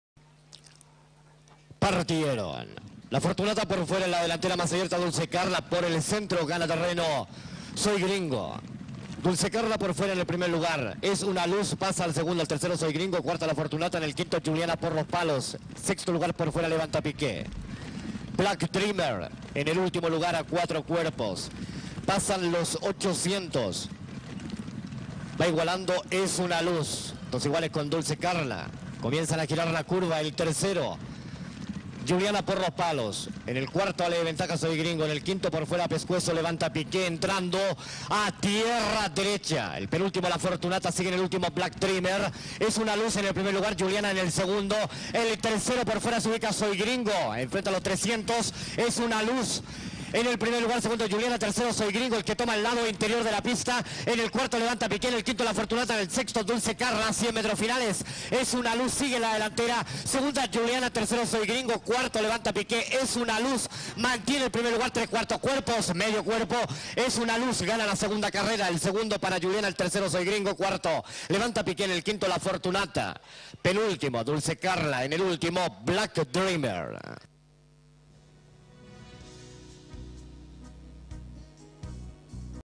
es una luz
ES UNA LUZ ganador 2ª carrera martes 21 de marzo 2017